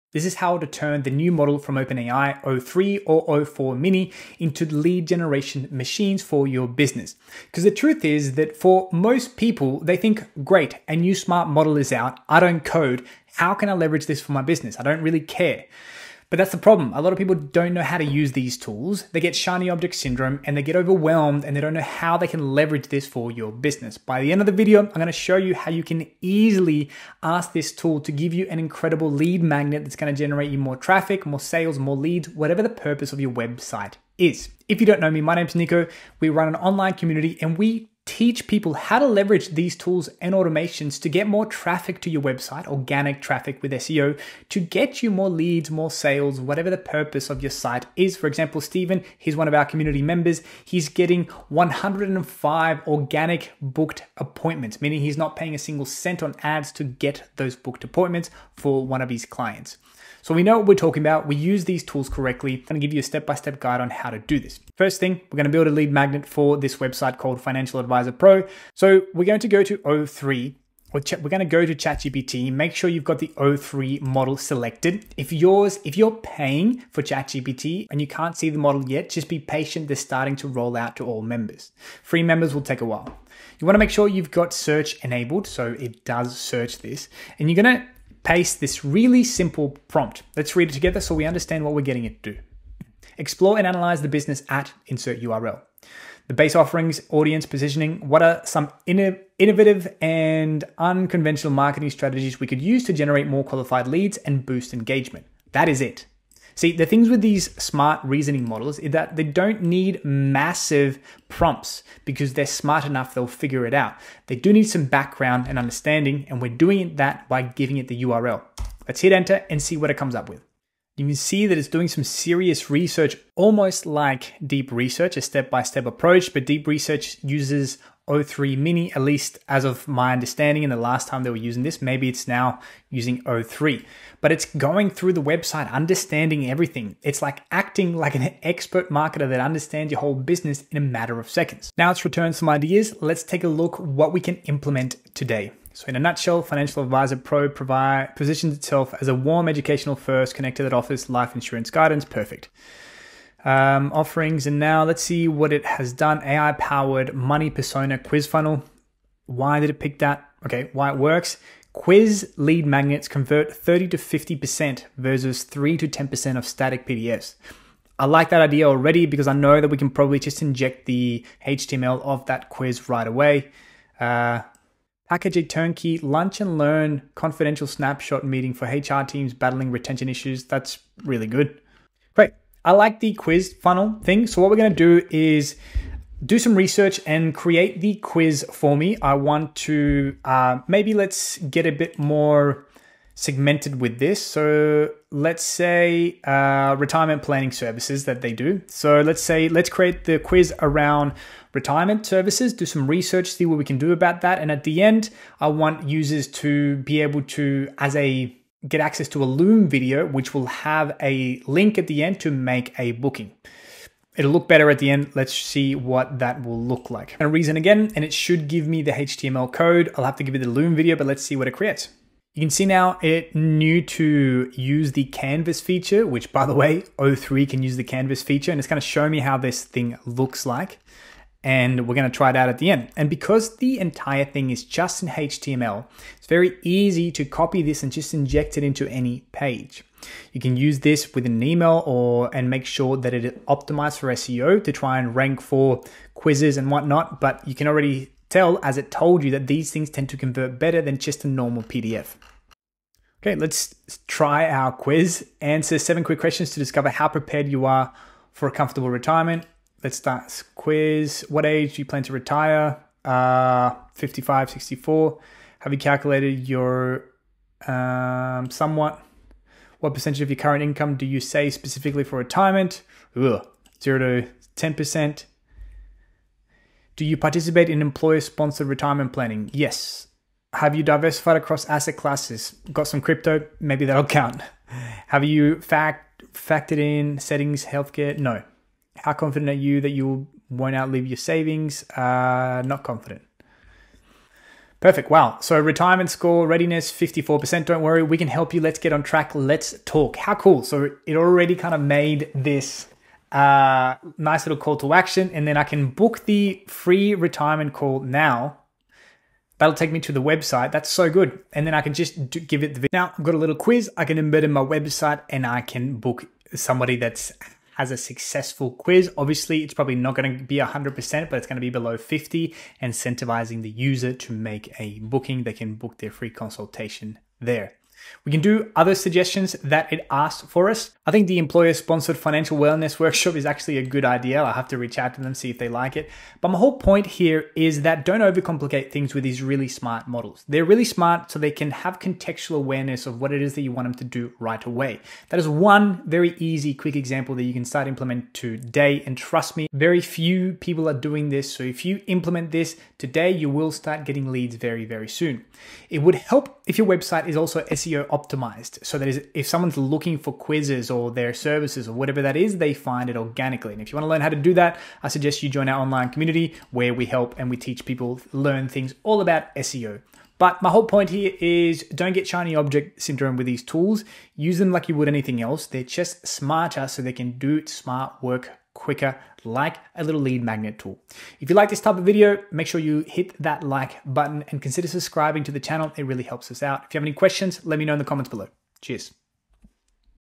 Turn OpenAI's New o3 Into an Endless Lead Machine (Step‑by‑Step)
Learn to use AI and Automations like this to Rank#1 on Google, Search GPT and Perplexity 👉🏼

PROMPT USED:
Explore and analyze the business at {INSERT WEBSITE}. Based on its offerings, audience, and positioning, what are some innovative or unconventional marketing strategies we could use to generate more qualified leads and boost engagement?